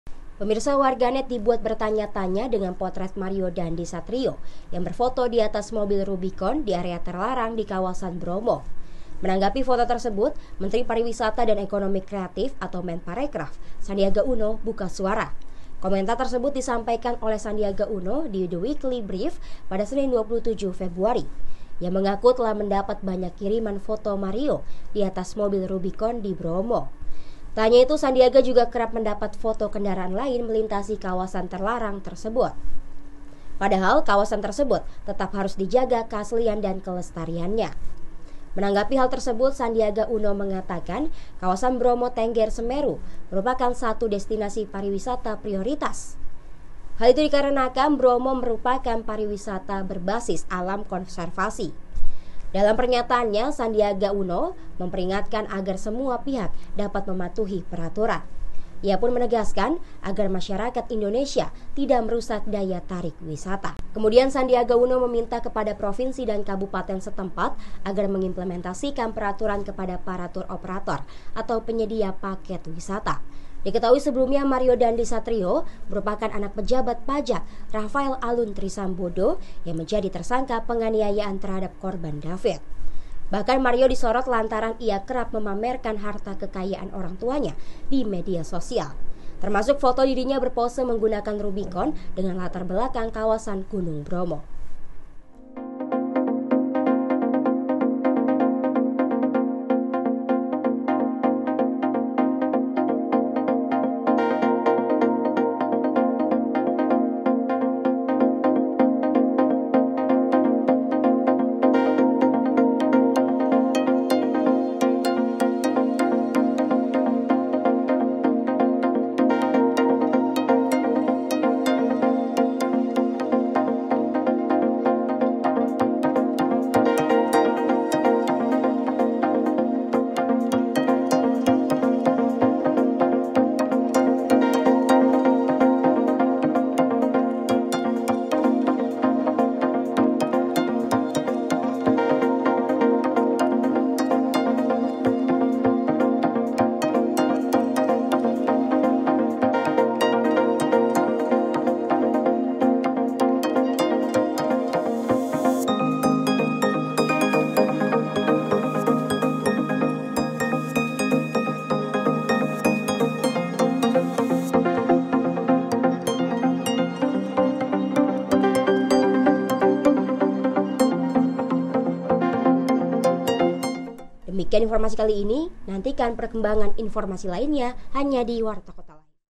Mario Dandy Pose dengan Rubicon Masuk di Kawasan Wisata Bromo, Sandiaga Uno Angkat Bicara
WARTAKOTALIVE.COM, JAKARTA - Warganet dibuat bertanya-tanya dengan potret Mario Dandy Satriyo yang berfoto di atas mobil rubicon di area terlarang di kawasan Bromo.

Menanggapi foto tersebut, Menteri Parisiwata dan Ekonomi Kreatif (Menparekraf) Sandiaga Uno buka suara.

Komentar tersebut disampaikan oleh Sandiaga Uno di The Weekly Brief pada Senin (27/2/2023).